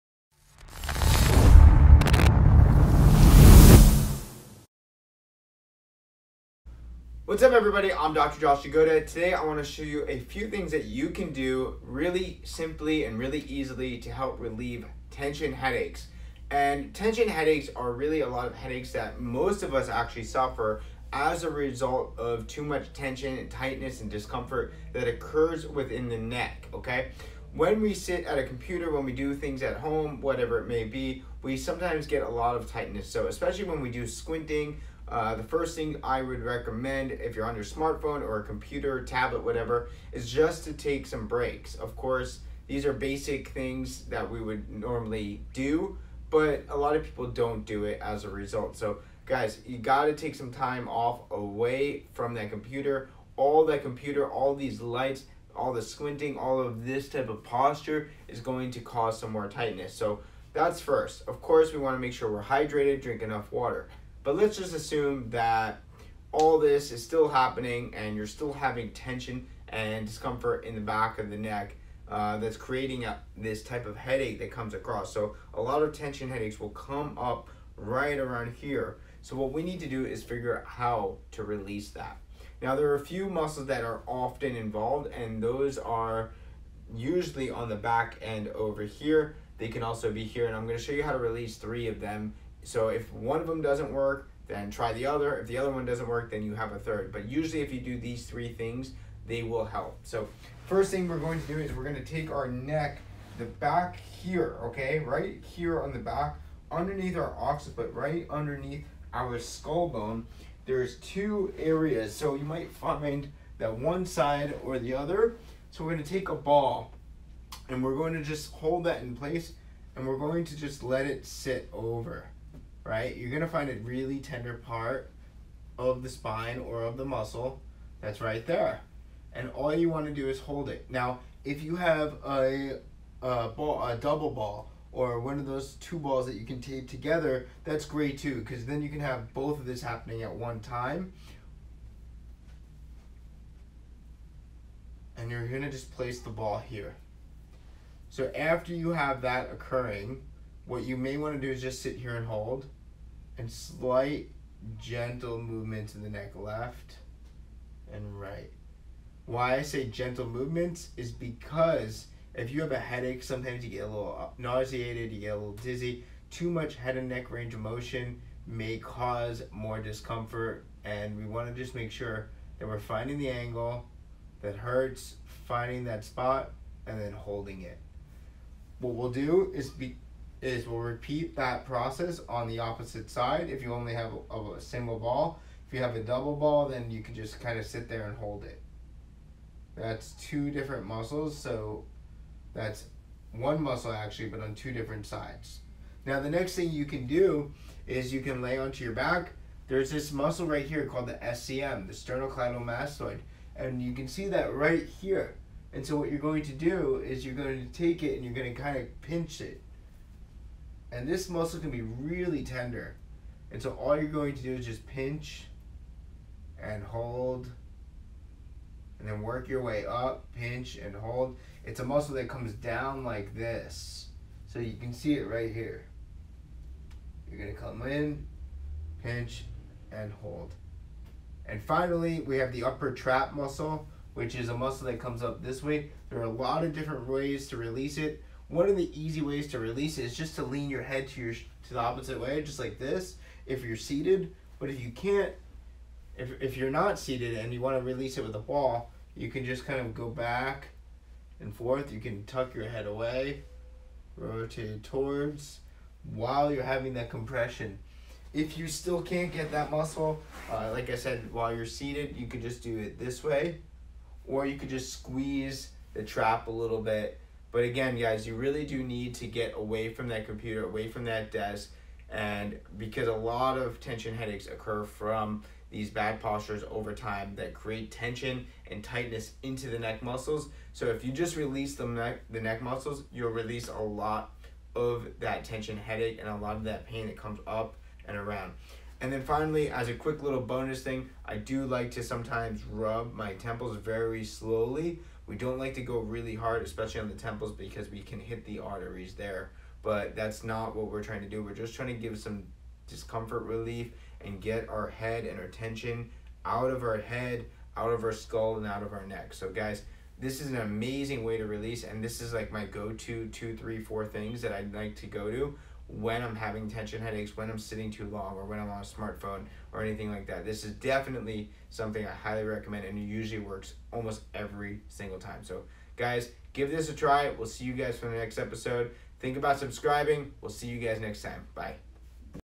How to Get Rid of Tension Headaches At Home
Get the FREE Low Back Pain Relief Guide:
to Get Rid of Tension Headaches At Home?

Tension headaches are the most common type of headache and they occur from overly tight and tense muscles that pull excessively.  Most people that feel tension headaches feel a "band" across their forehead and pressure.  In addition, much of this pressure, and tension is taken off when the affected muscles have been relieved of their tightness.